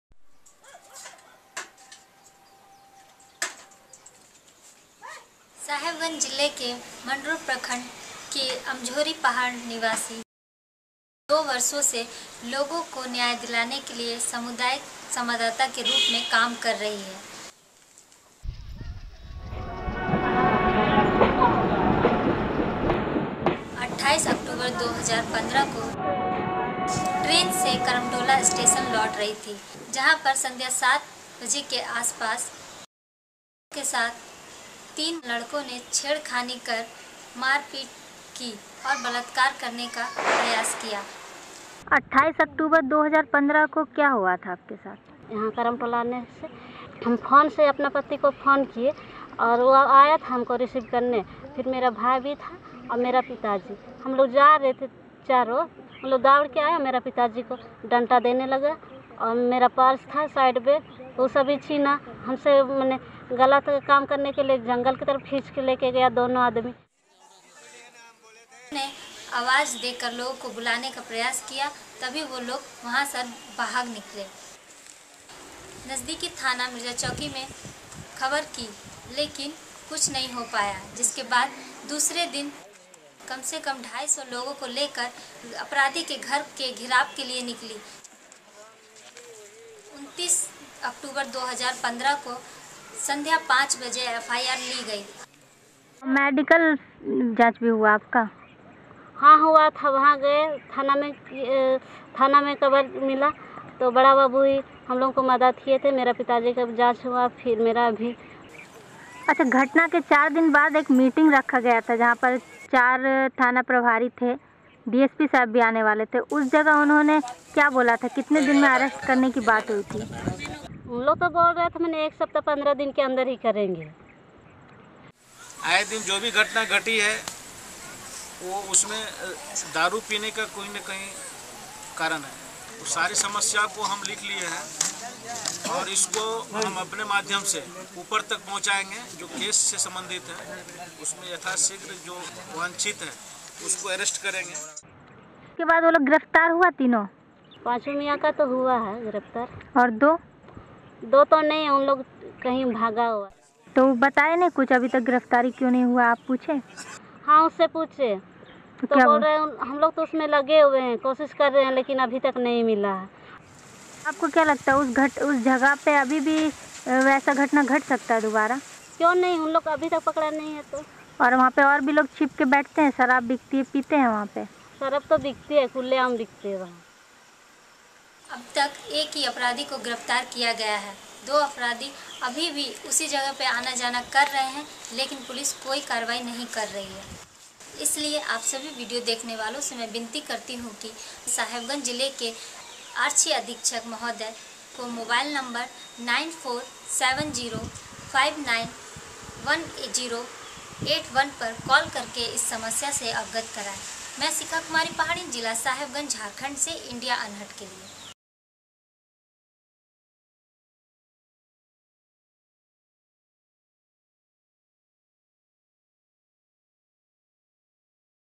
Survivor of abuse awaits justice
Shikha Kumari Pahadin reports from Sahibganj, Jharkhand for Video Volunteers.